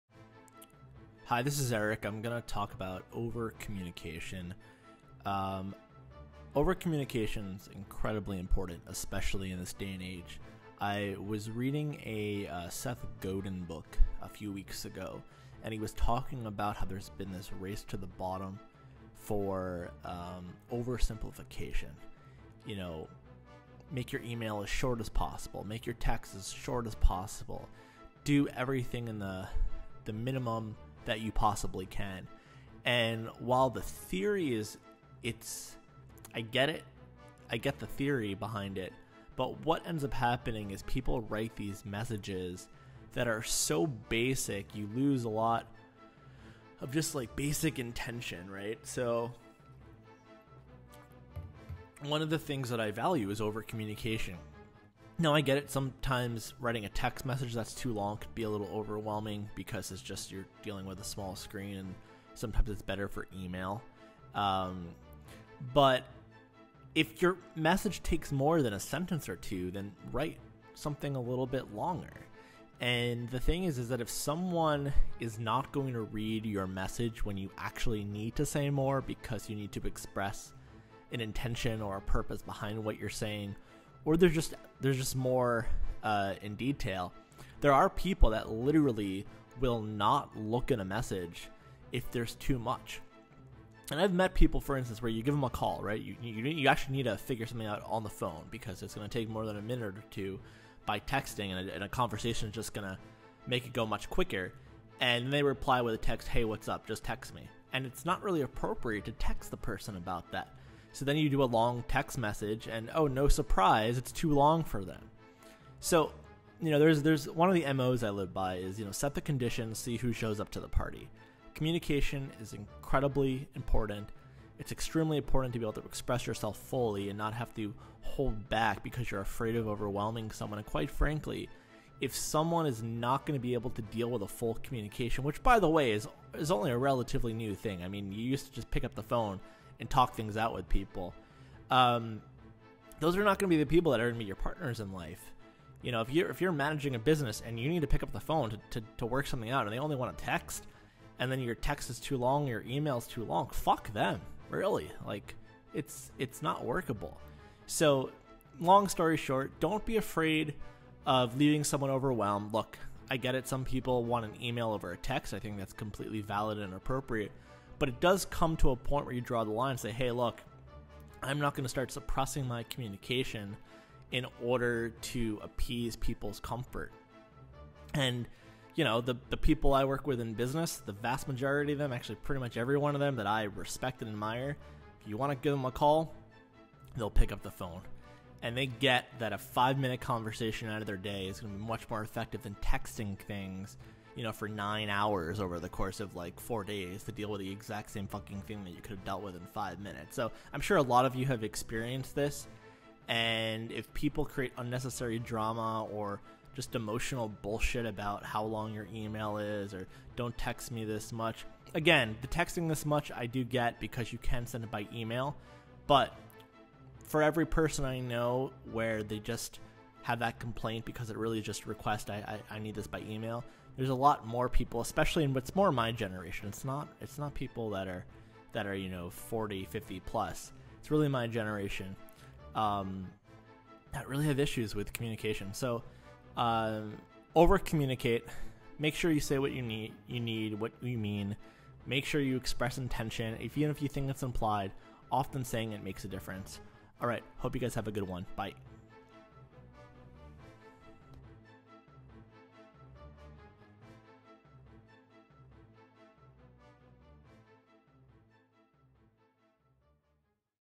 The Importance Of Over-communication | Eric Schleien
Eric Schleien discusses the importance of overcommunication in an age where there's been a race to the bottom in simplicity and cutting down communication under the guise of efficiency.